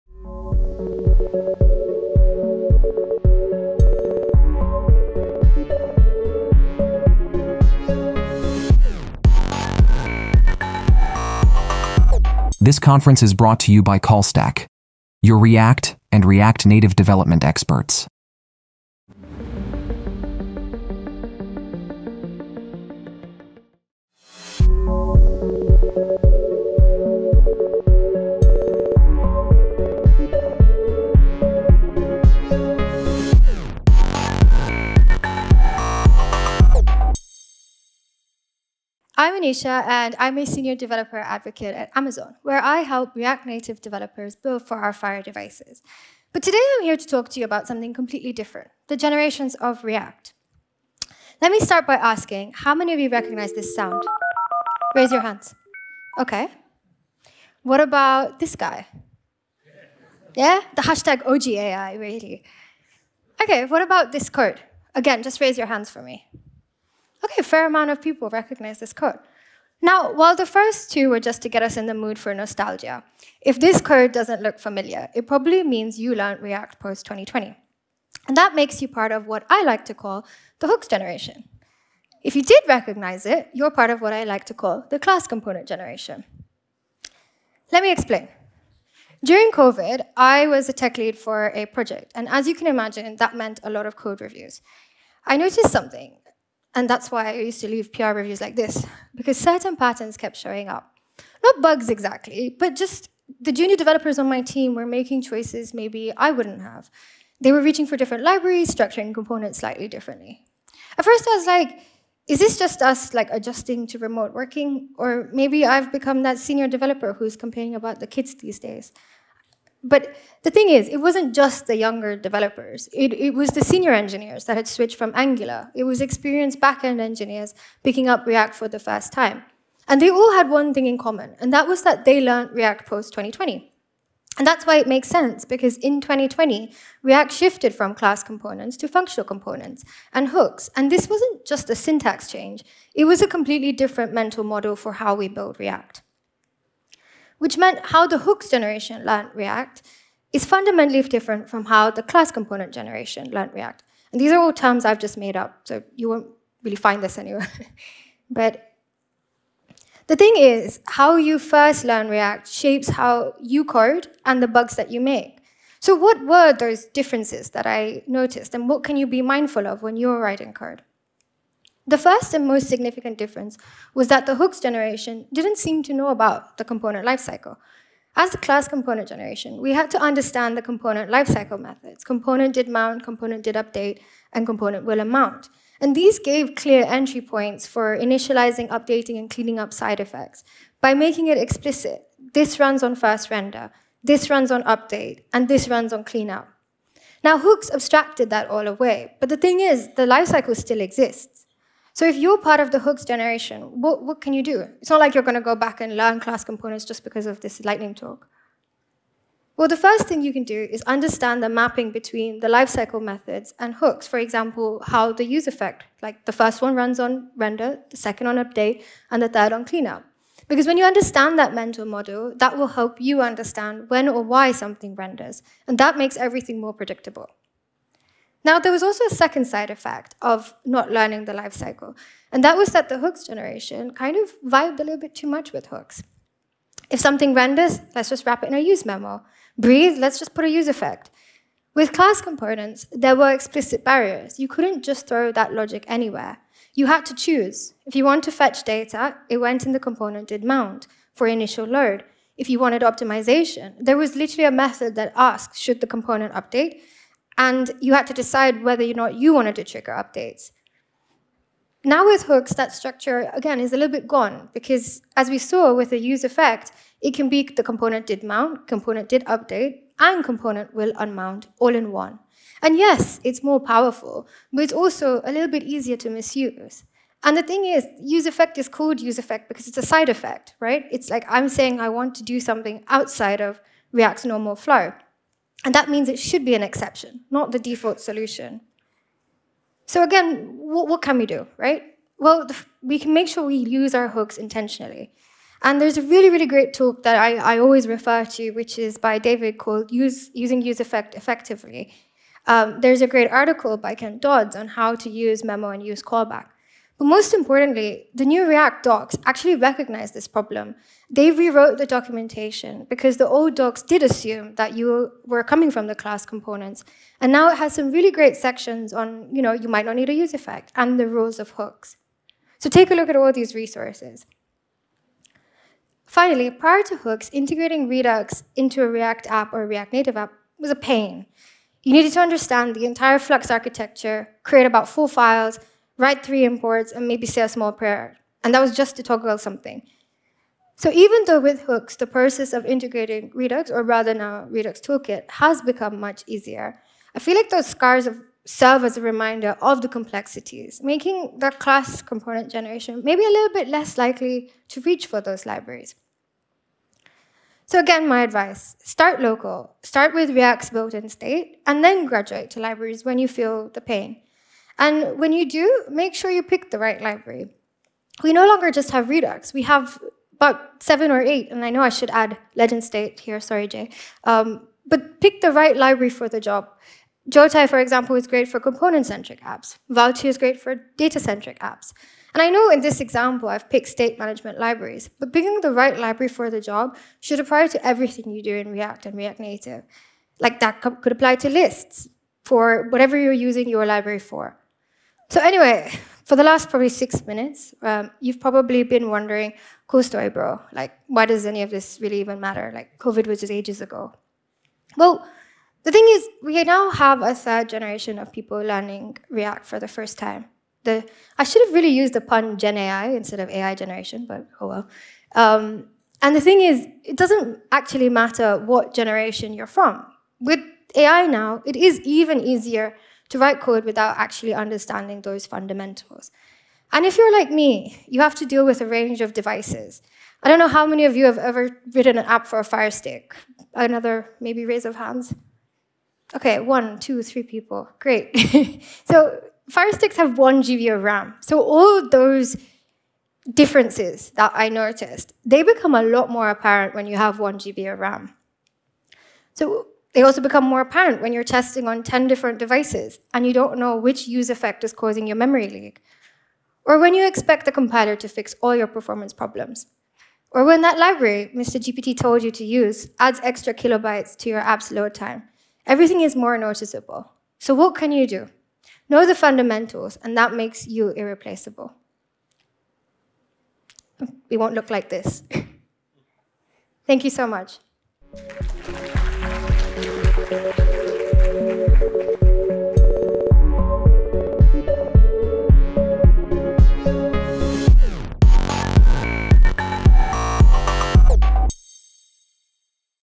The Generations of React | Anisha Malde at React Universe Conf 2025
React has been around for over a decade, and how you first learned it shapes how you code today.

At React Universe Conf 2025, Anisha Malde (Developer Evangelist at Amazon) explores the “generations of React” and how different learning journeys influence developer habits and best practices.

Skip the intro ⏩ 0:39

🔹 The Class Component Generation: understanding lifecycle methods and the discipline of explicit structure.
🔹 The Hooks Generation: learning React post-2020 with new flexibility, but also new pitfalls like overusing `useEffect` and `useMemo`.
🔹 The Compiler Generation: a new wave of developers leaning on AI and compiler optimizations, sometimes without grasping fundamentals.
🔹 How differences in starting points affect state management choices, library preferences, and performance trade-offs.
🔹 Why knowing the fundamentals of React’s lifecycle and mental models makes you more effective; especially on constrained devices like Fire TV.

Whether you started with classes, hooks, or compilers, this talk is a reminder that fundamentals never go out of style.

Follow React Universe Conf on X to stay up to date 🐦